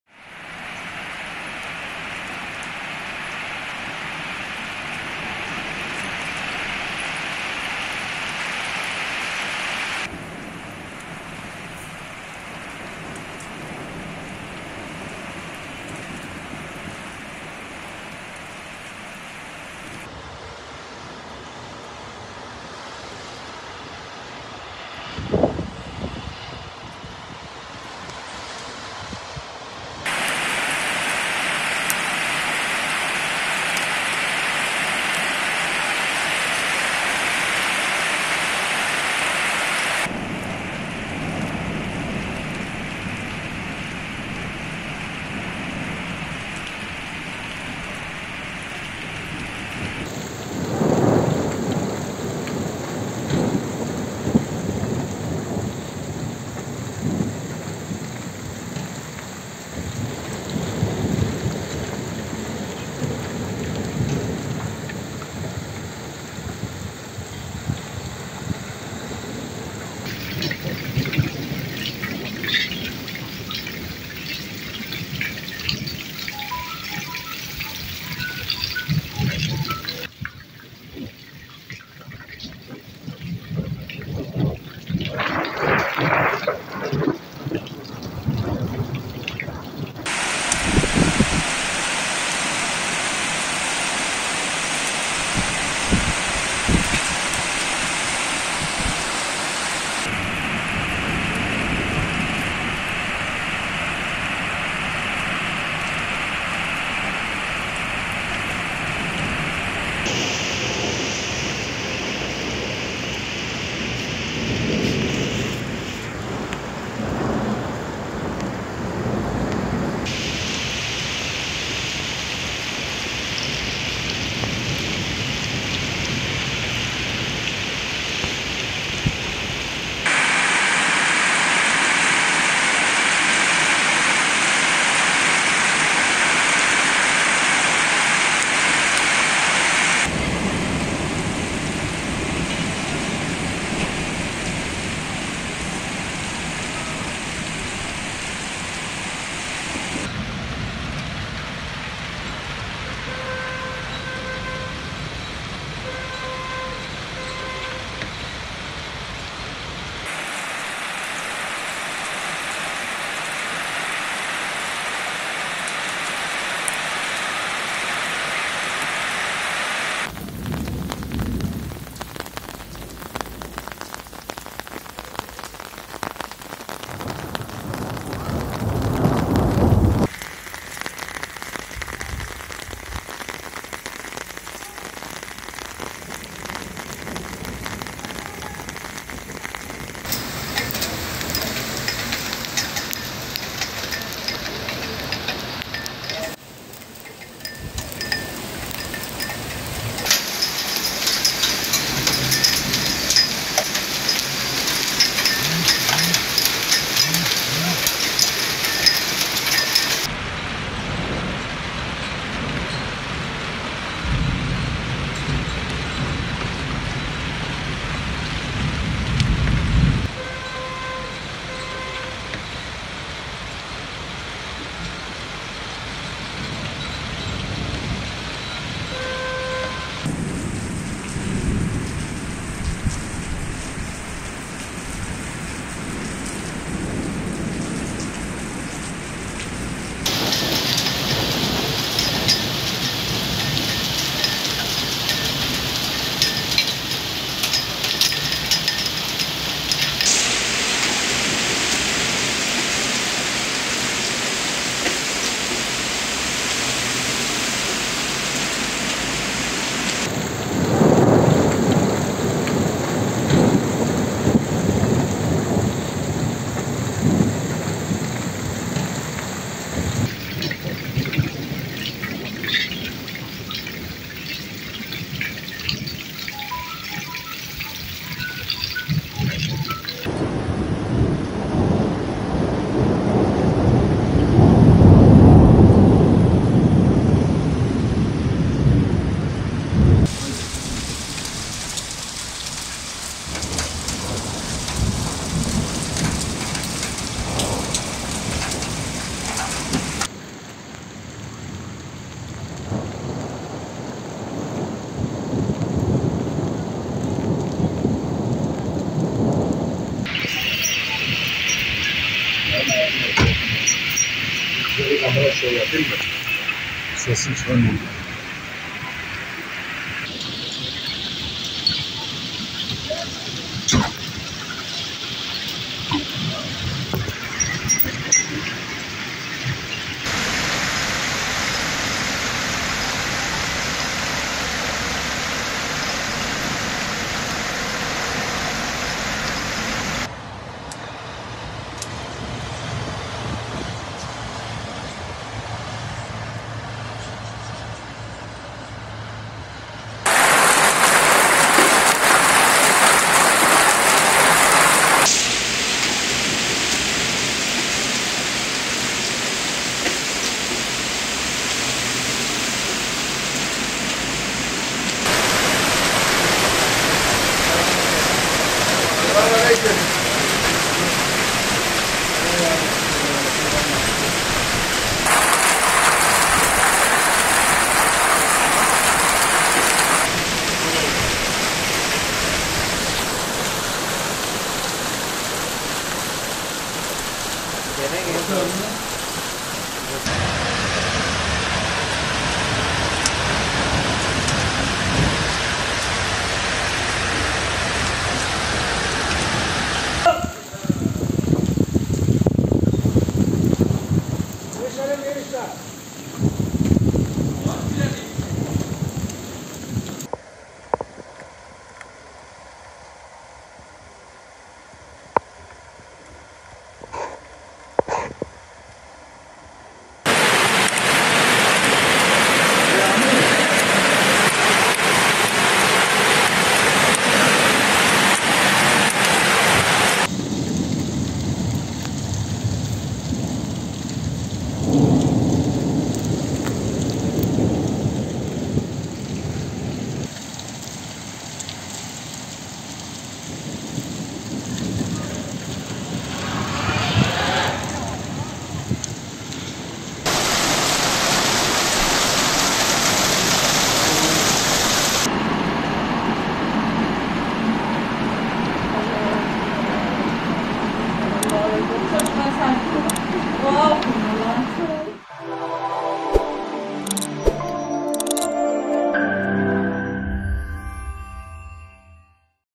Gaziantep'te ceviz büyüklüğünde dolu yağışı
Gaziantep'te aniden bastıran ve kısa sürede etkili olan sağanak yağış ile dolu, hayatı olumsuz etkiledi.

Değerli İzleyicilerimiz;